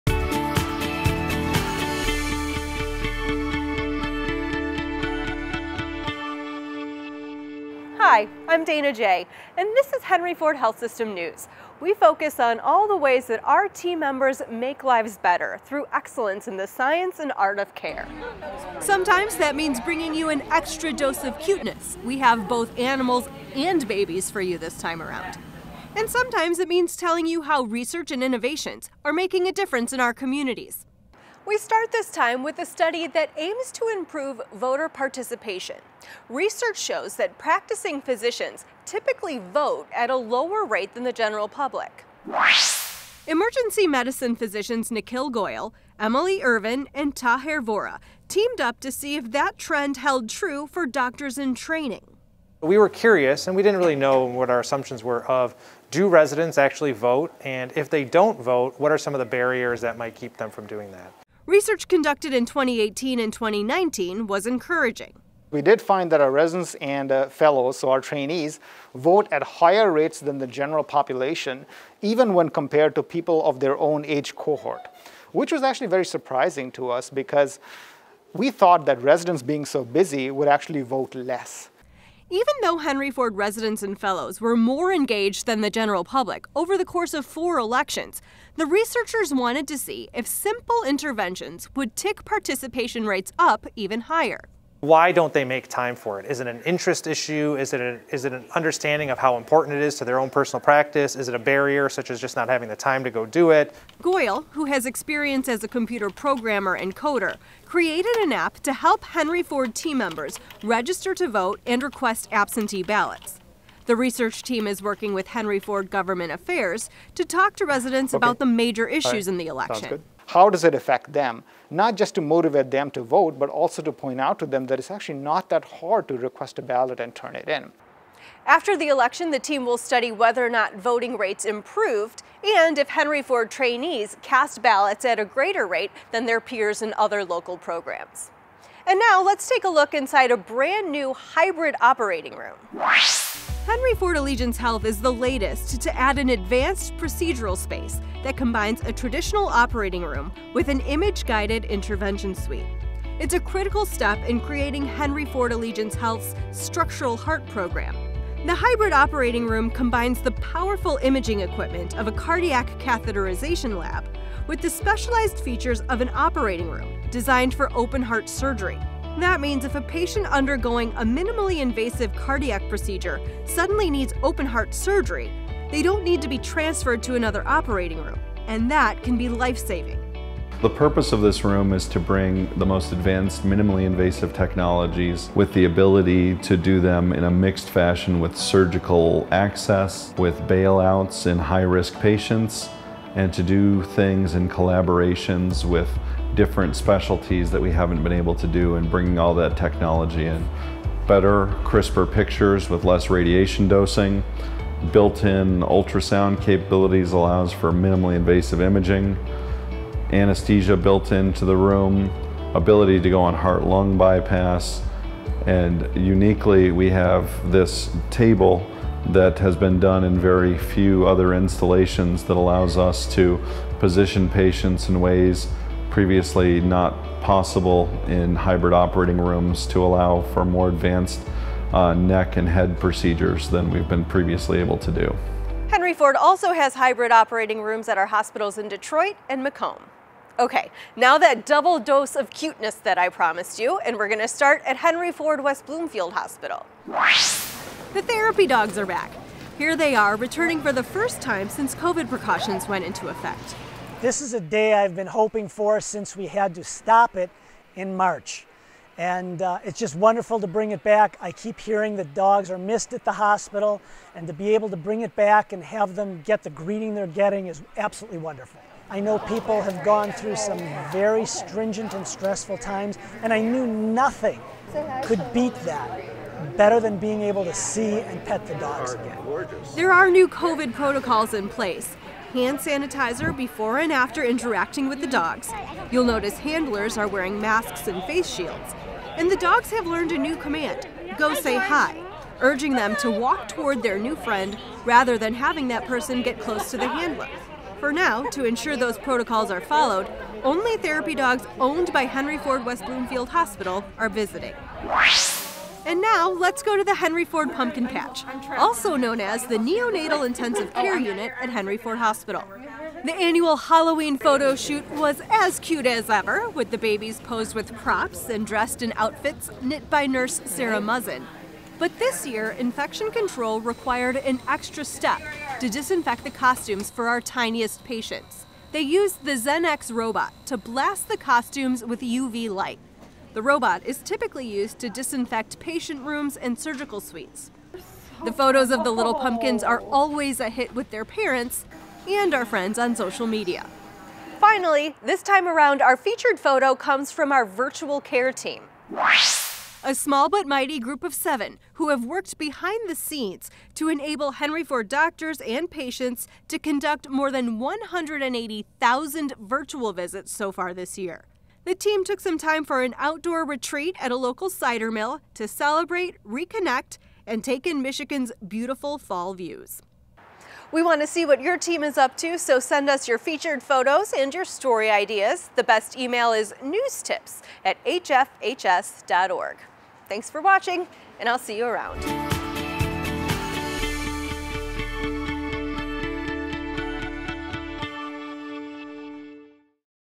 Henry Ford Health System News 10/21/20
Henry Ford Health System News brings you stories about how our team members are making lives better through excellence in the science and art of care.
Skip to the Stories:
:32 Emergency Medicine Physicians Aim to Improve Voter Participation
2:18 Henry Ford Allegiance Hospital Opens State-of-the-Art Hybrid O.R.
4:00 Therapy Dogs Return to Henry Ford West Bloomfield Hospital
5:05 NICU Nurses Arrange Halloween Photo Shoot